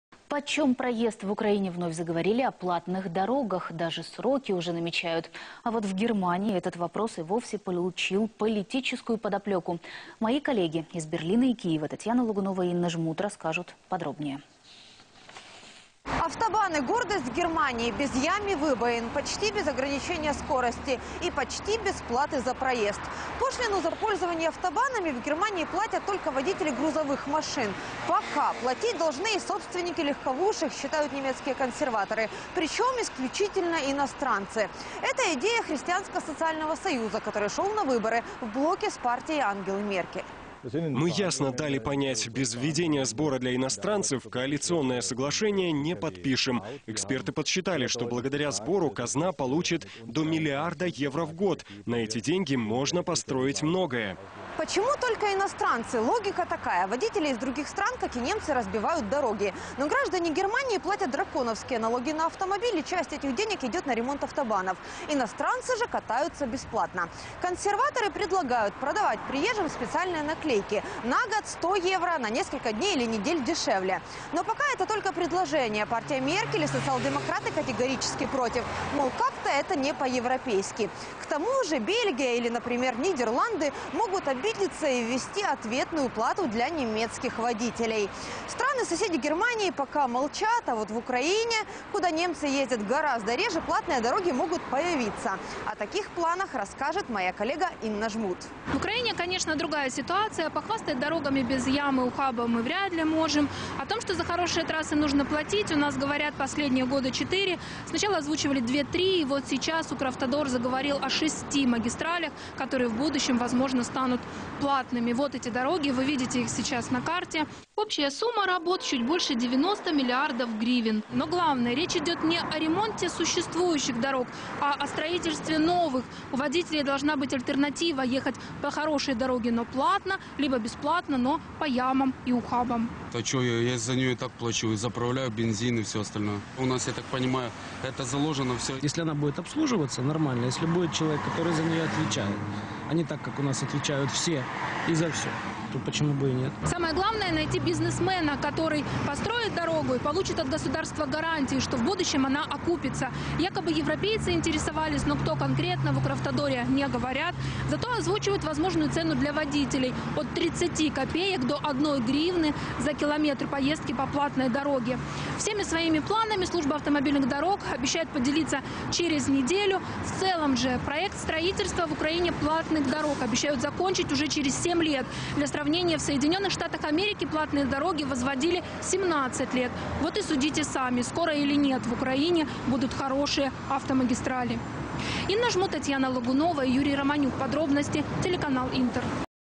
В Украине могут появиться шесть платных дорог
В Украине могут появиться шесть платных дорог - Новости. Вечер - Интер - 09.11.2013

Ukraine may receive six toll roads - news. Evening - inter - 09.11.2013